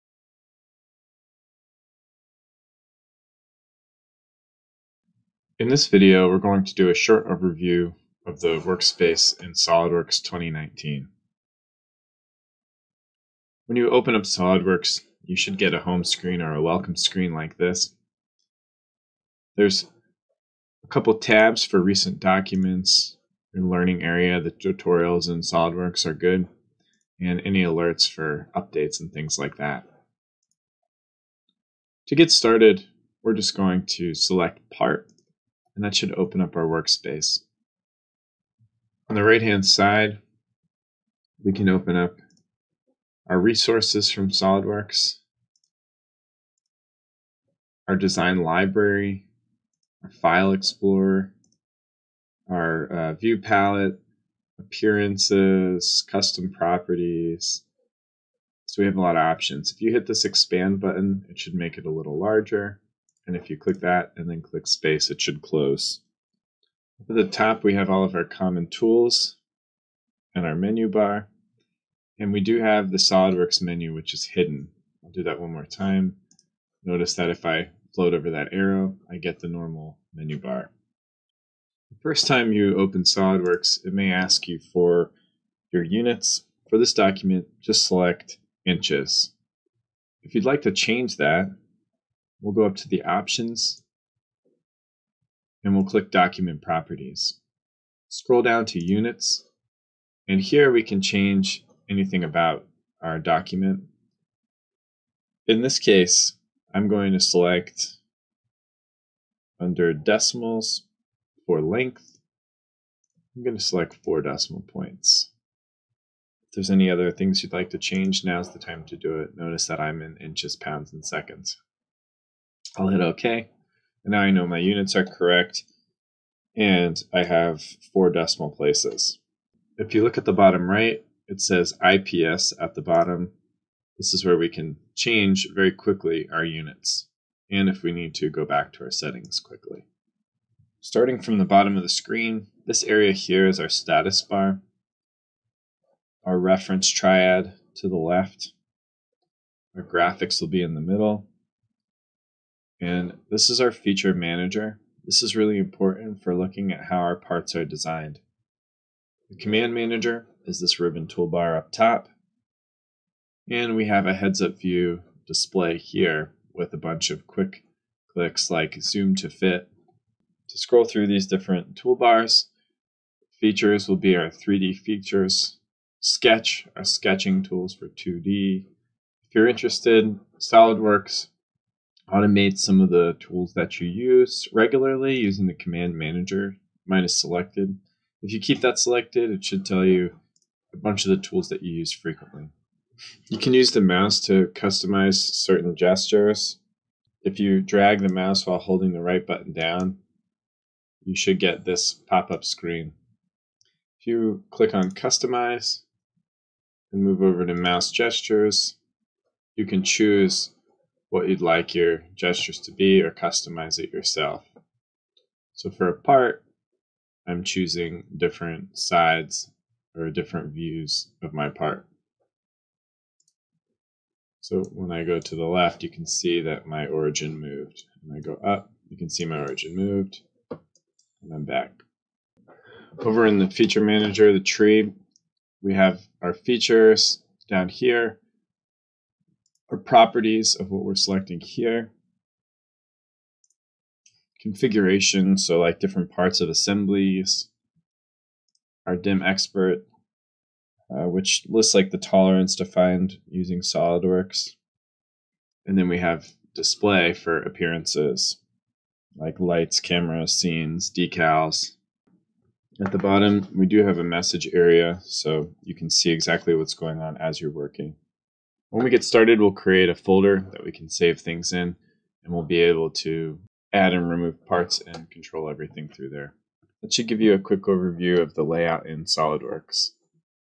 SOLIDWORKS 2019 Intro and Overview of Workspace
This is a short video showing off the workspace and overview of SOLIDWORKS 2019.